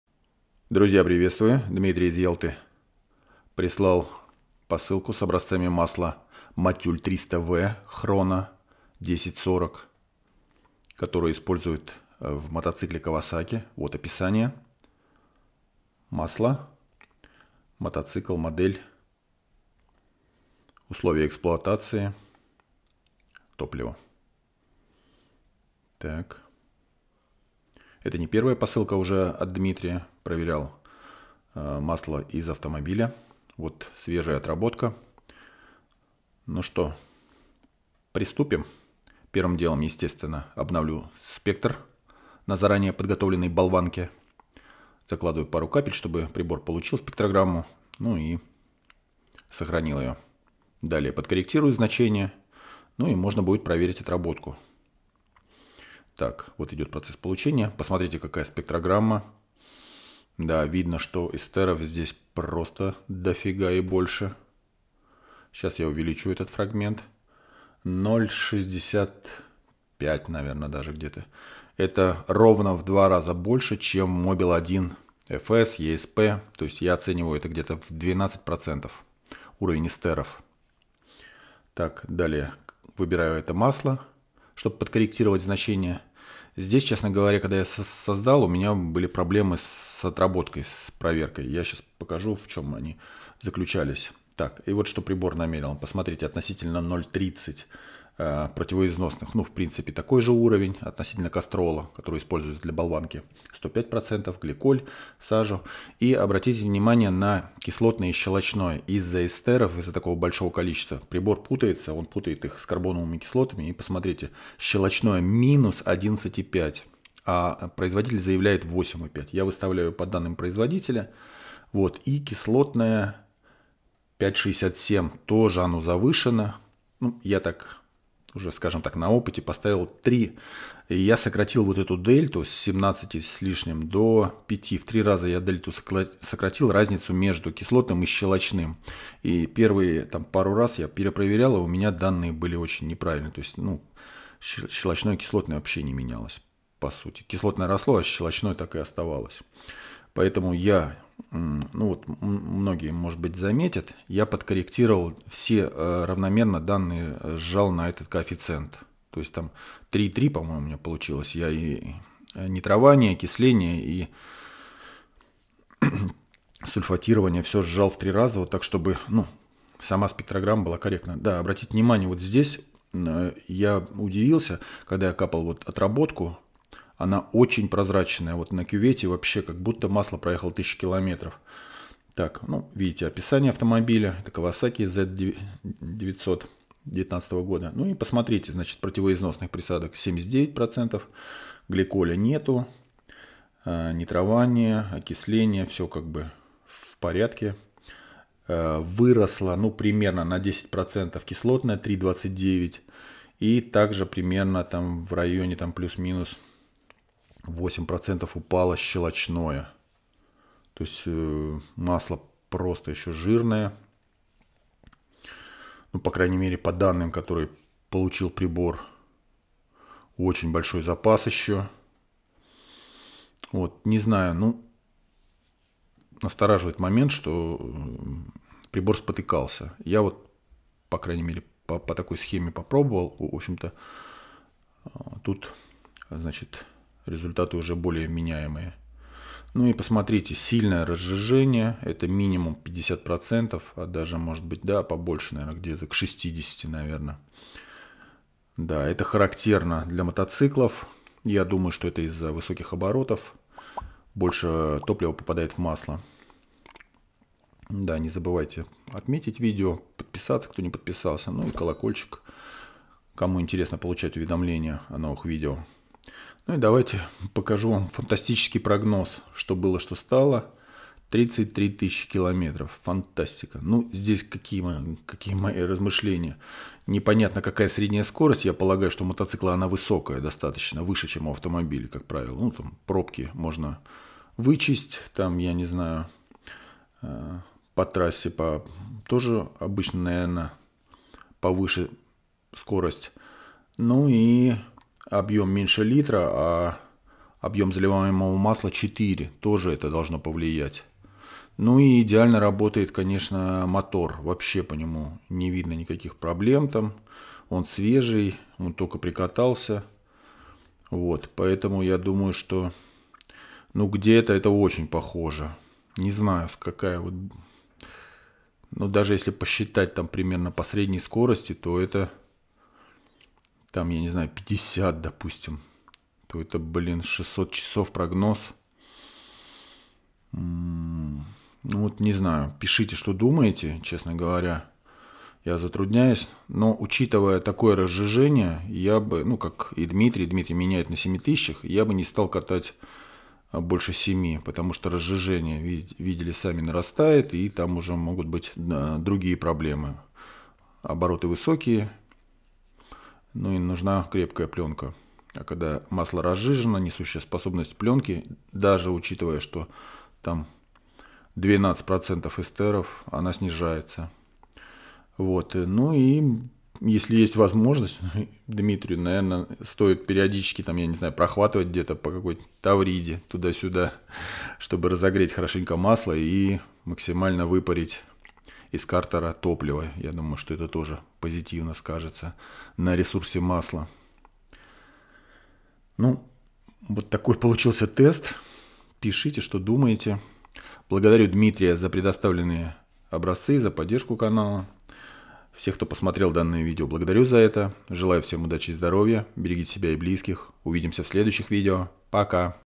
Motul 300V Chrono 10w40 (Kawasaki Z900, 5 000 km)
Дмитрий из Ялты, прислал  образцы свежего масла Motul 300V Chrono 10w40
Допуски и соответствия данного масла, заявлены производителем как "ABOVE EXISTING MOTORSPORT STANDARDS" (ВЫШЕ ДЕЙСТВУЮЩИХ СТАНДАРТОВ ДЛЯ АВТОСПОРТА)

Используется в мотоцикле:
Kawasaki Z900, 2019,
двигатель: 950 см. куб., 125 л.с.
объем масляной системы - 4 литра.
с общим пробегом около 15 000 км.

Отработка с пробегом 5 000 км.

Топливо Ультра (95)

Благодарю Дмитрия, за образцы и поддержку канала!